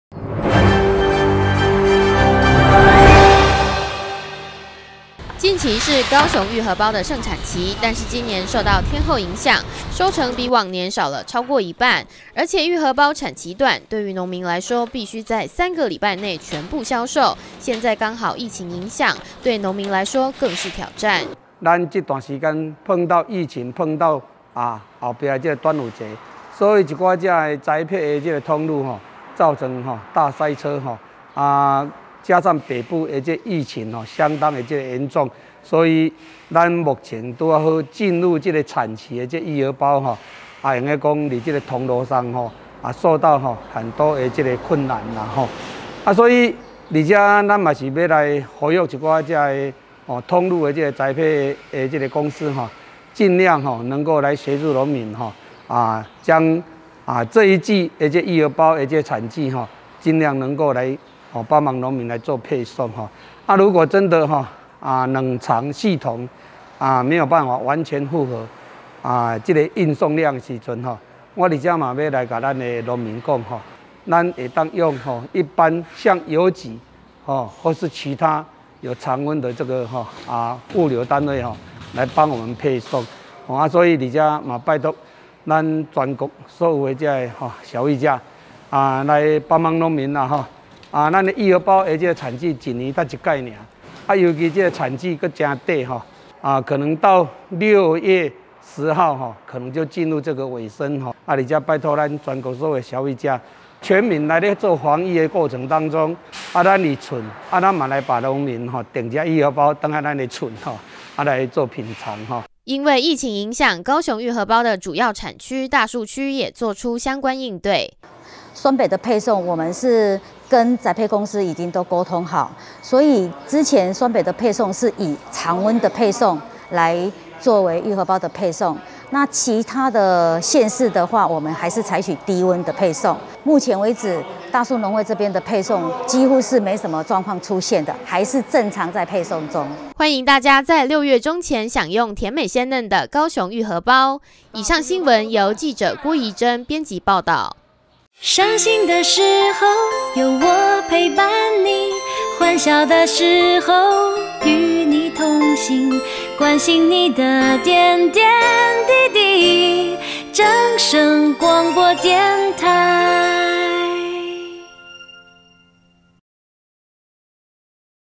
【CSNEWS​】高雄玉荷包正當季 幫你安心宅到家/CS看正聲
大樹玉荷包，宅配好方便
正值玉荷包荔枝產季，雖然疫情肆虐
但是不影響玉荷包的品質，端午送禮最佳選擇
就是來自高雄大樹的玉。荷。包